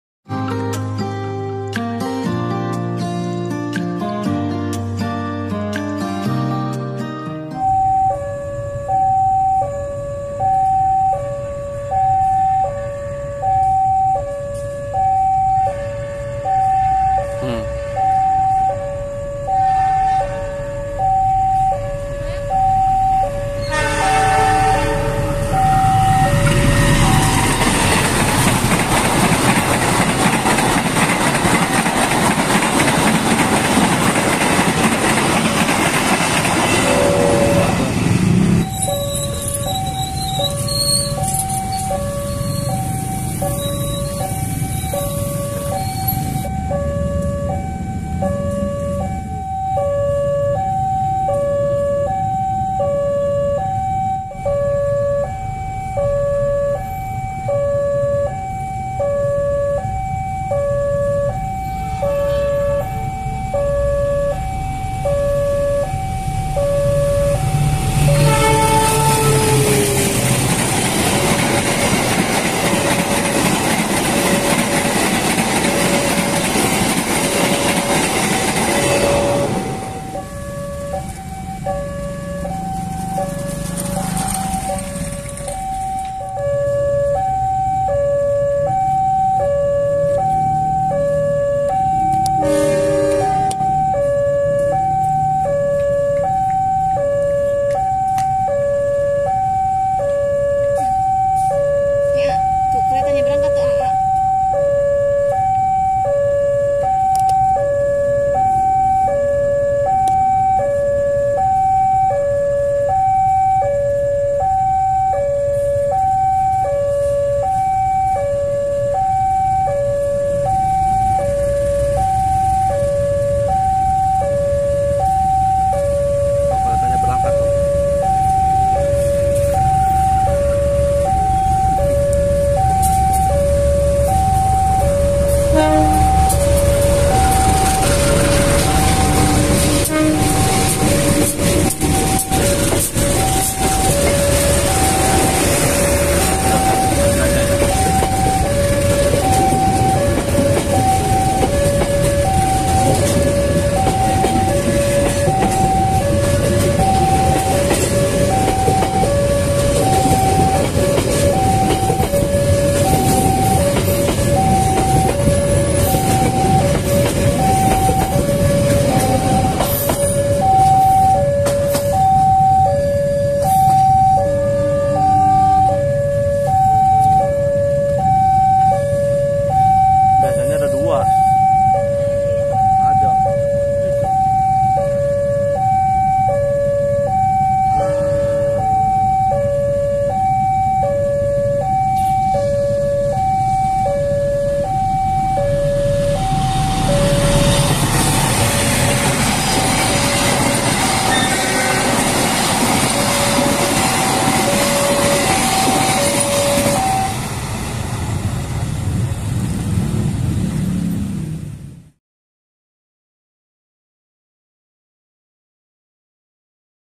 2 KALI DISUSUL APA NGGAK NYESEK TUHH || niat hati mau cepat sampai malah, yasudah lah
Kereta api lokal Walahar malam di susul 2 kali oleh KAJJ.